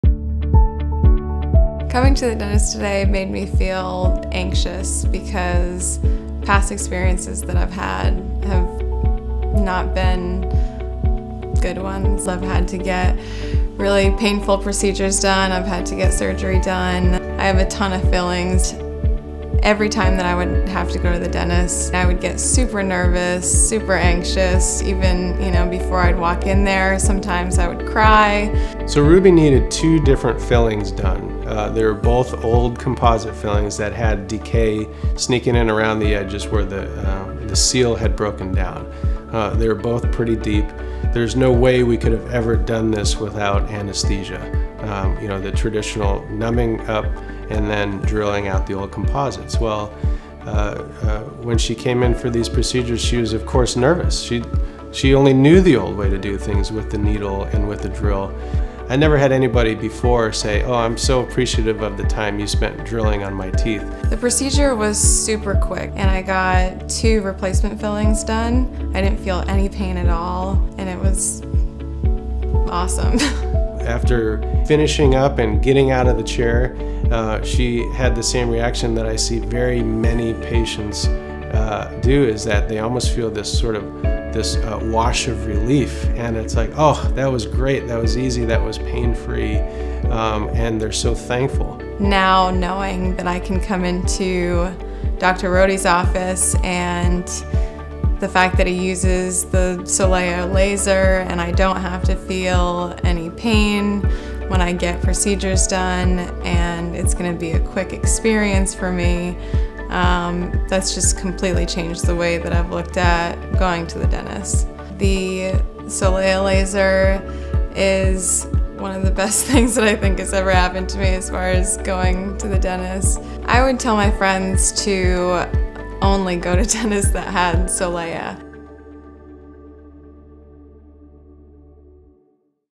Dr. Jeffrey Rohde Solea (All-Tissue) Dental Laser Patient Testimonial - Ruby's Experience
Ruby has had terrible experiences with previous dentists. When she came to Dr. Rohde, he used Solea for her cavity preps. She was blown away by the speed and lack of pain.